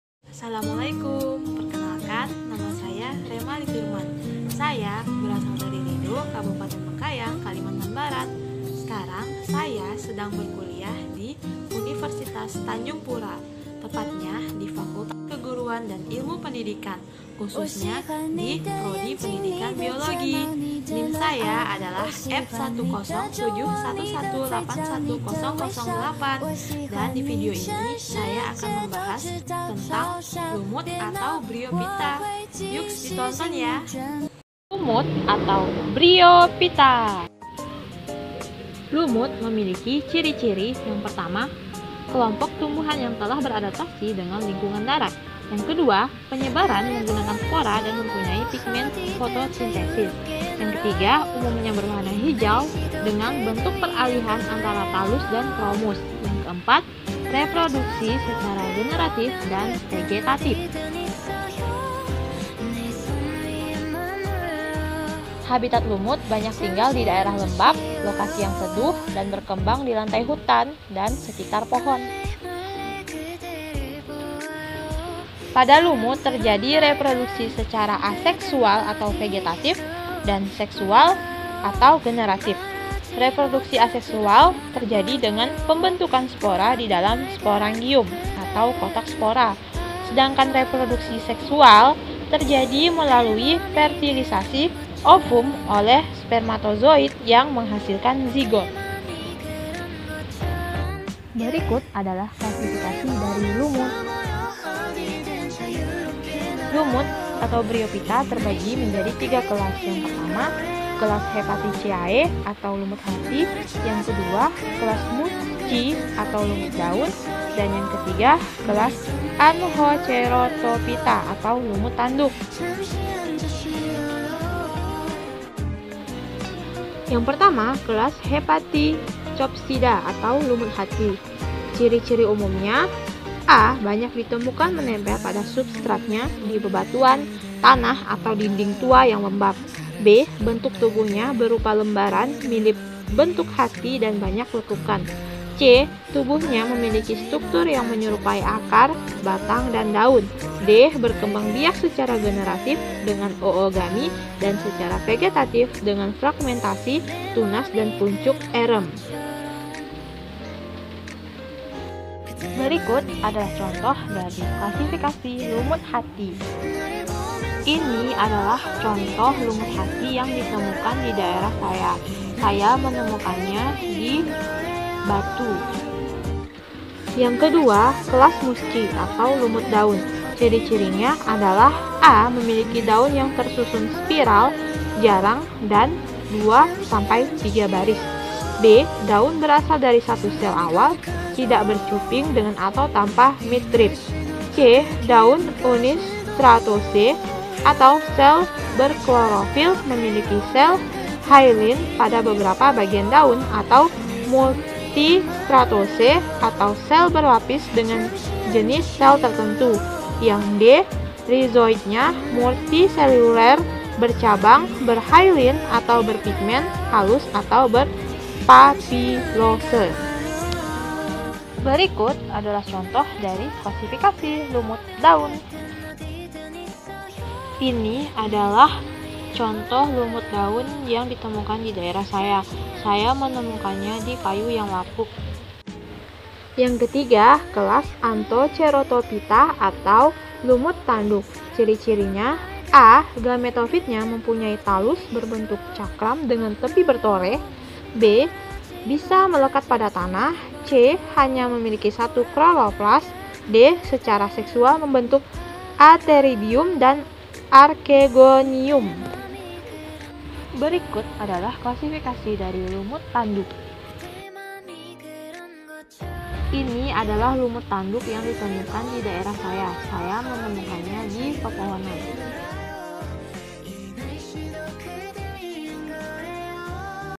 Yukss belajar tentang lumut
Lumut sering kita jumpai di sekitar kita, tetapi kita tidak mengetahui kalau lumut itu berbeda beda

Dengan video ini semoga bermanfaat ya

Follow @relef.im